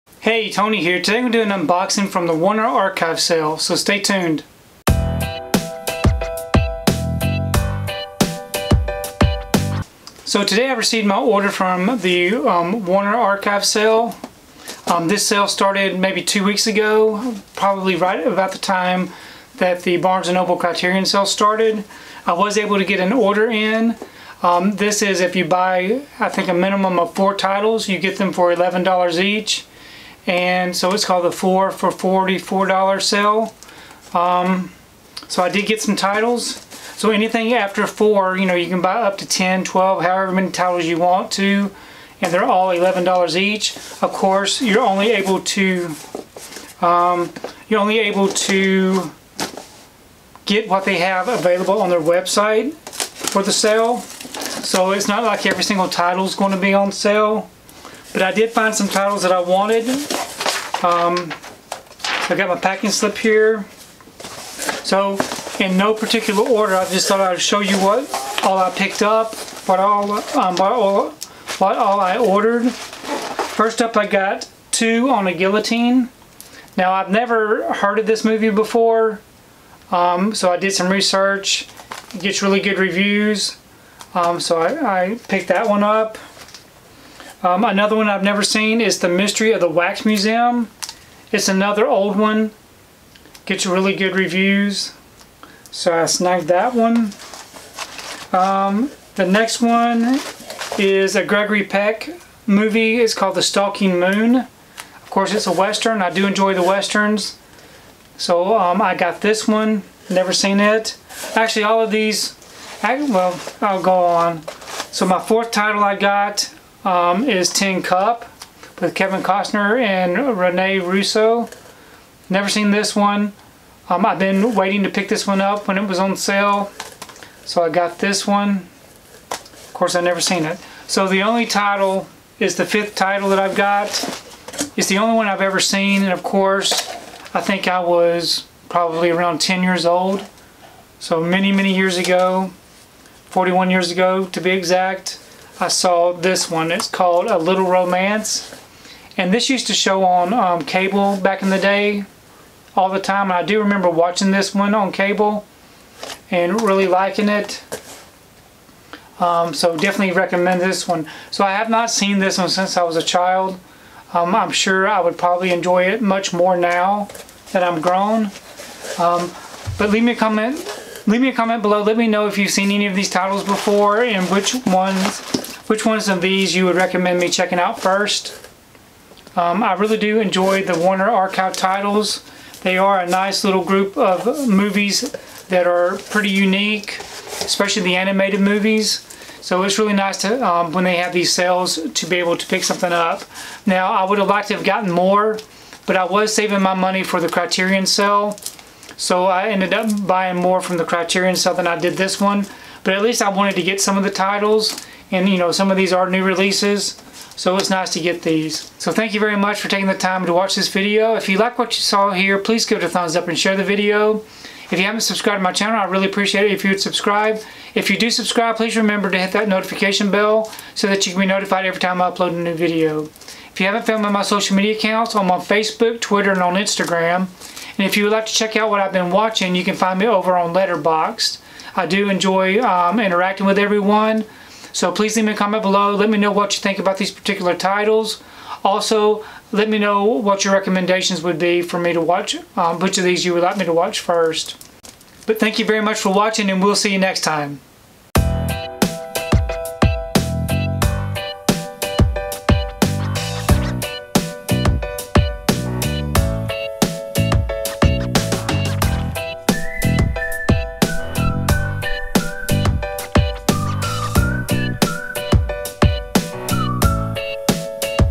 Warner Archive Blu-ray Haul - 5 Titles Added to the Collection
Check out my Warner Archive Blu-ray Haul -  5 Titles Added to the Collection!

Two On A Guillatine
The Mystery of the Wax Museum
The Stalking Moon
Tin Cup
A Little Romance

These are the bags I use to protect my Steelbooks and Slipcovers: